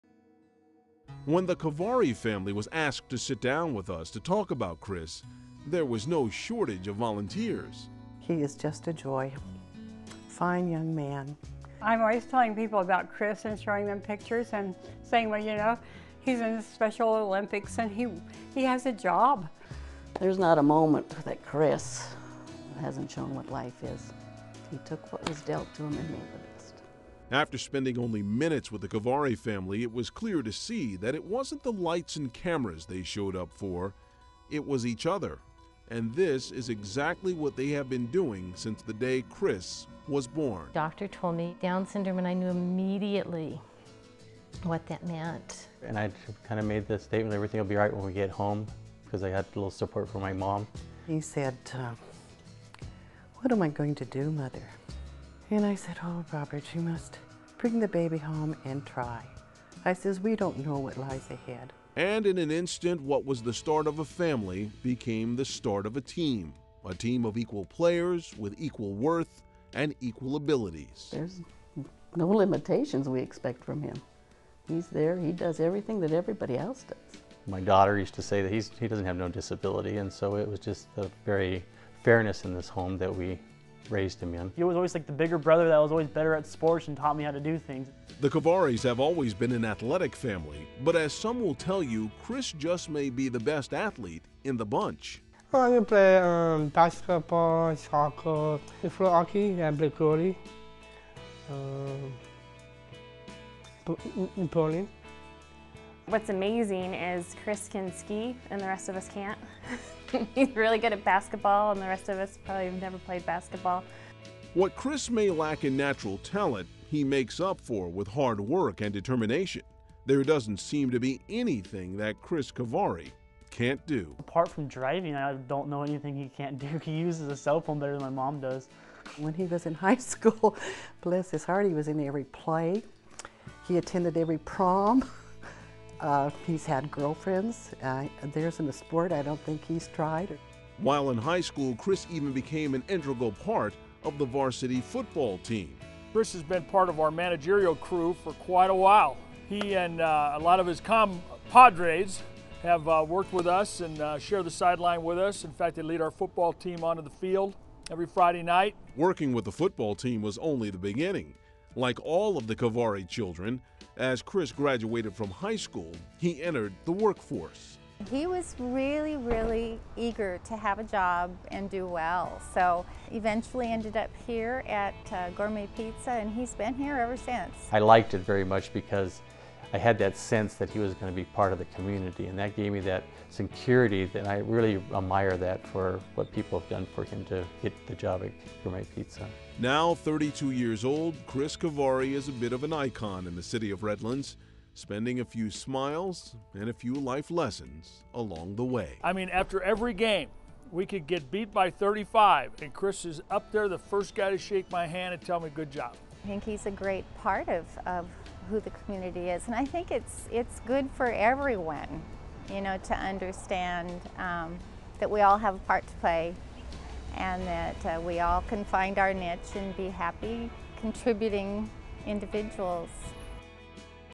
No Limitations - Chris Kevari
Chris Kevari was lucky to be born into not just a family, but a team.  From day one, Chris, born with Down syndrome, has been an integral part of his supportive family.

His family says that he is the best athlete in a family of sports lovers.  He plays basketball, soccer, floor hockey, and bowling, just to name a few.  He also is the only member of his family that can ski!

Chris now has a job at Gourmet Pizza in Redlands.  His friends and family say that he is an icon in the city of Redlands and is a big part of who the community is.

Support athletes like Chris Kevaria by volunteering for Special Olympics